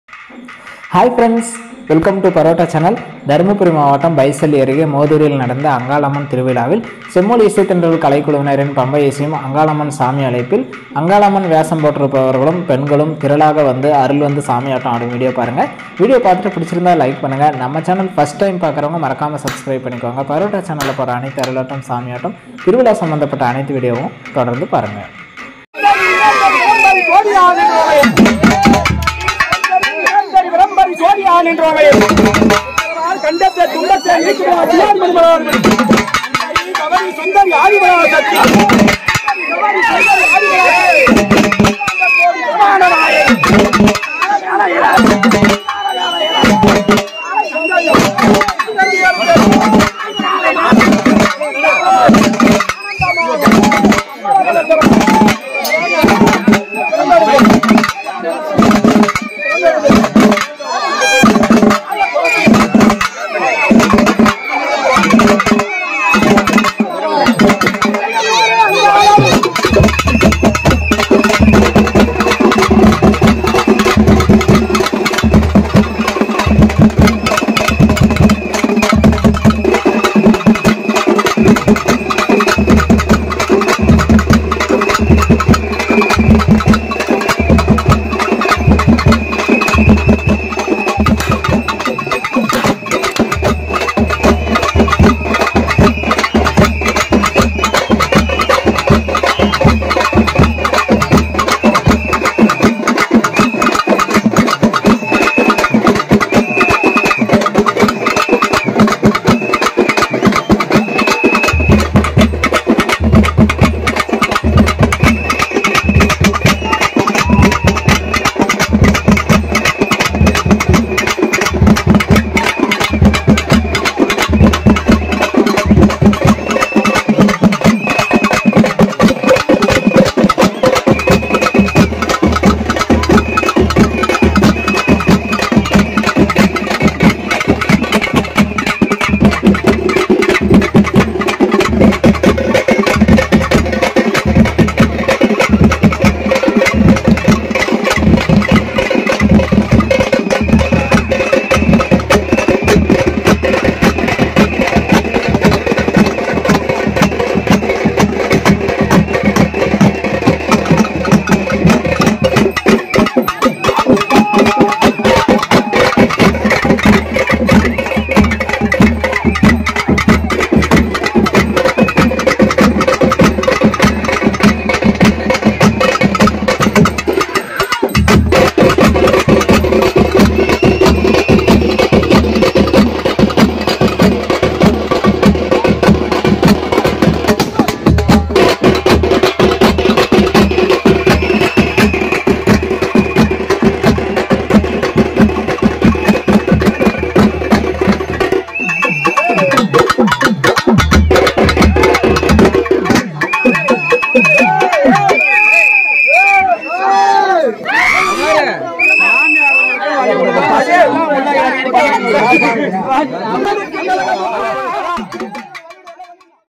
அங்காளம்மன் சாமி அழைப்பில் பெண்கள் சாமி ஆட்டம் | Sami Attam | Parotta Channel.
தினப் புகழ் கலைக் குழு தருமபுரி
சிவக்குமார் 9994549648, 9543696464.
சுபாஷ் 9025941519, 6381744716